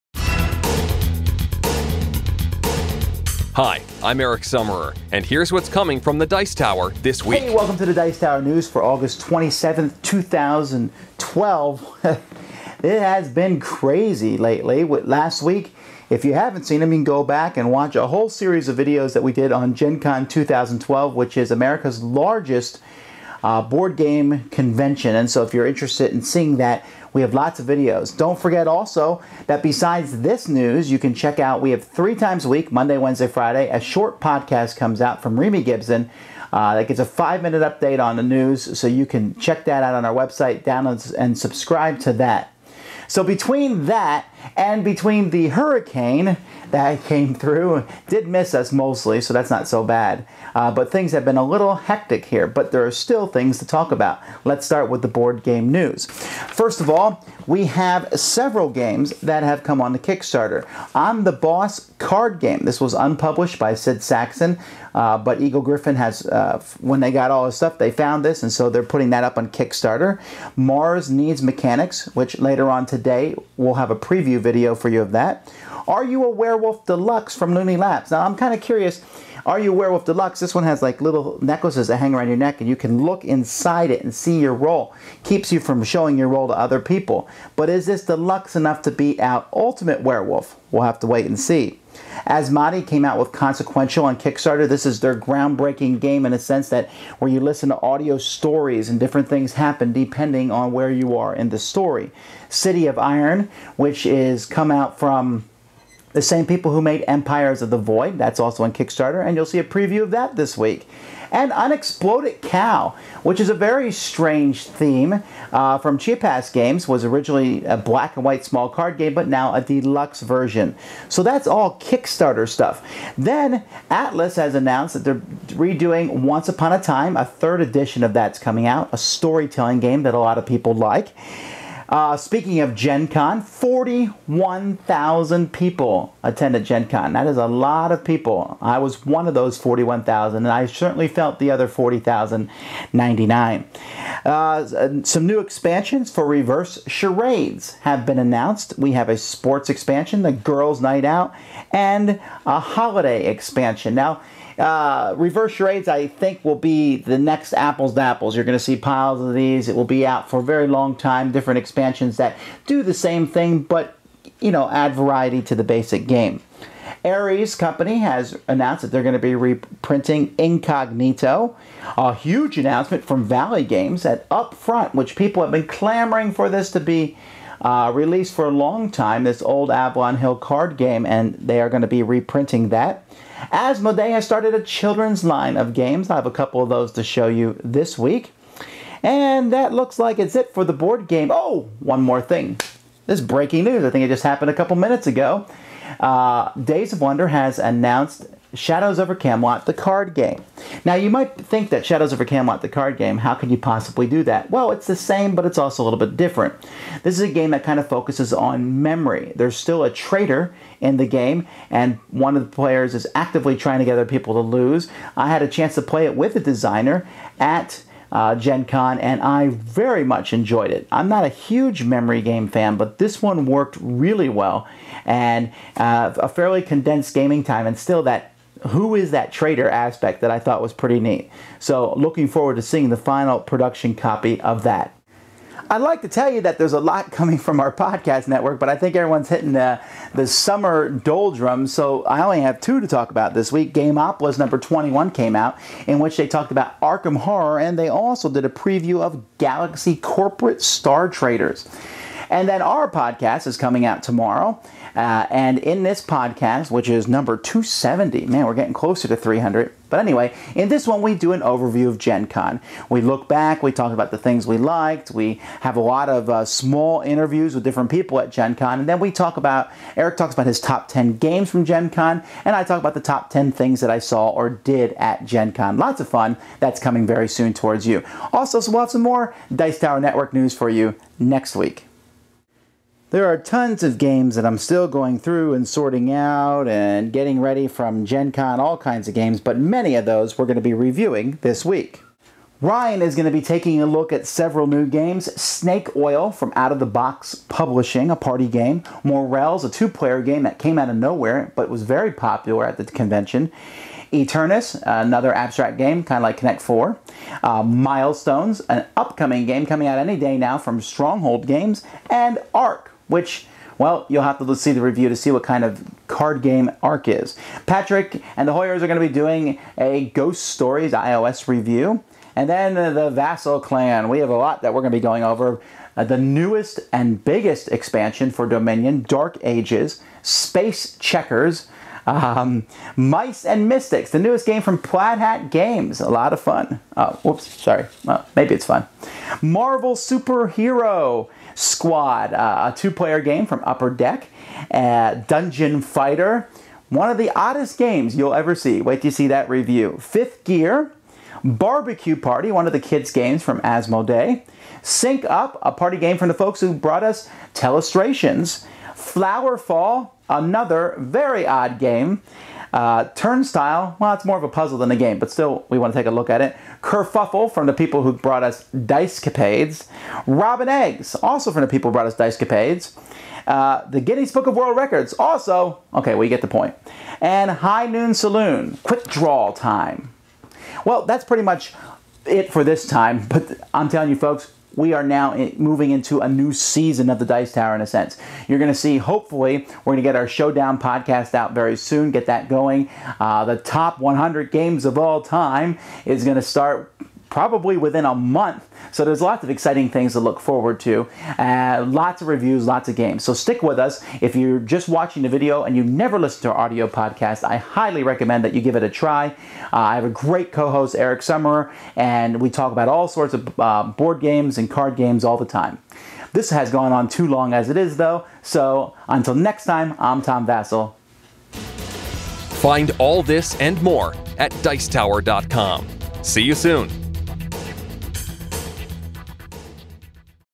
Dice Tower News August 27, 2012
Tom Vasel looks at board game new